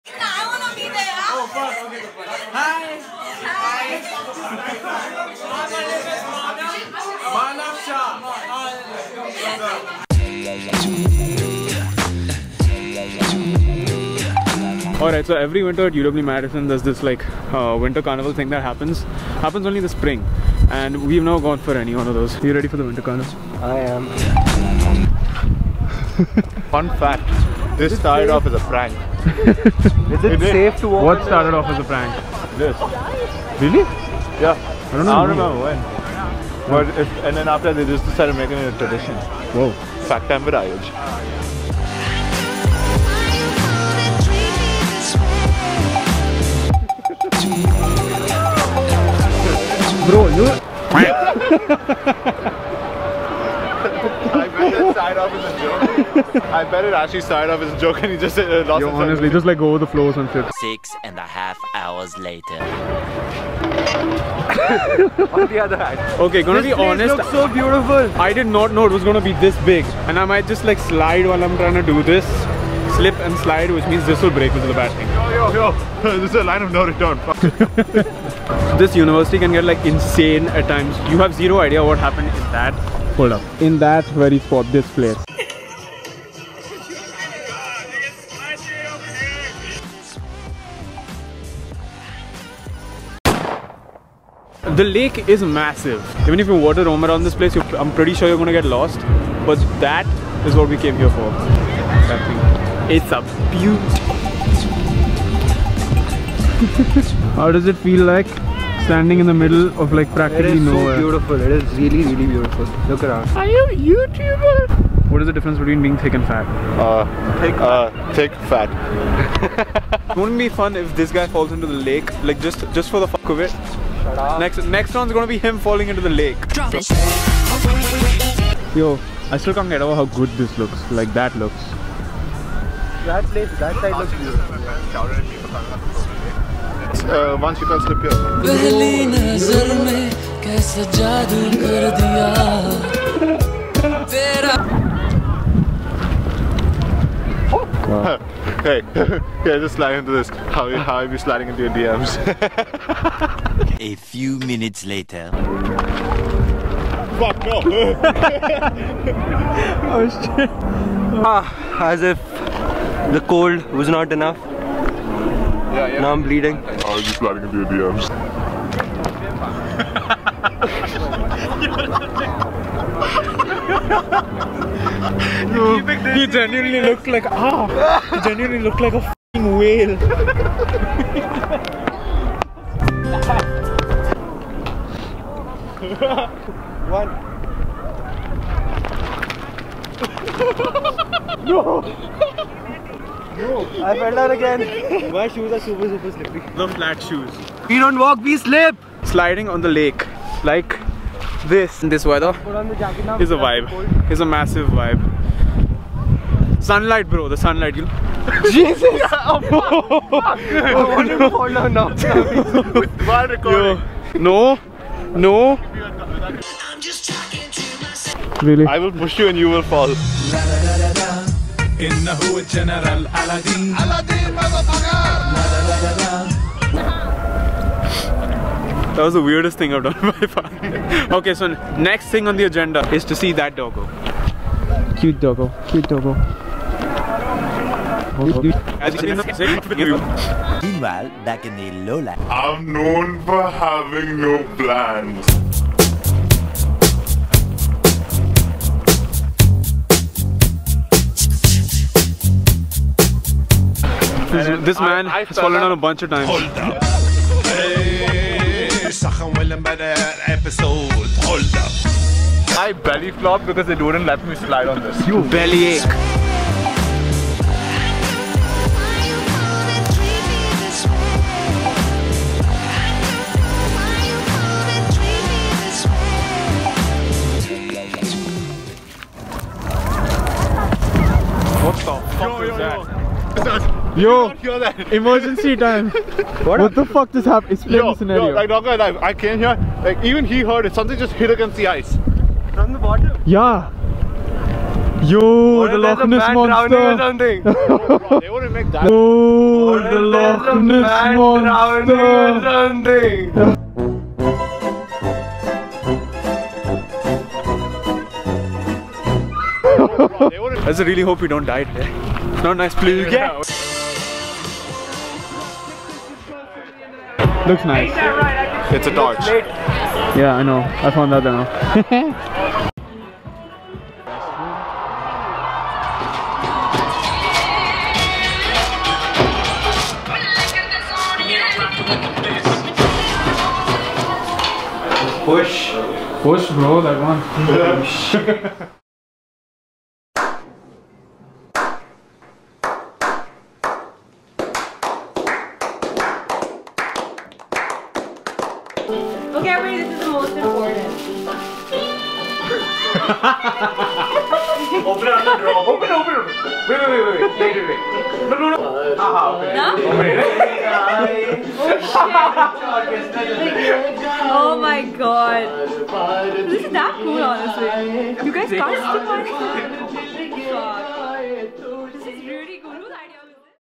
A Day In The Life - Winter Carnival at UW Madison
Welcome to a day in the life of the lads going to the Winter Carnival on Lake Mendota. Turns out it's actually quite fun slipping on snow 🤷‍♂️. Also, here's the Saturdays Are For The Boys S2 Epilogue :)))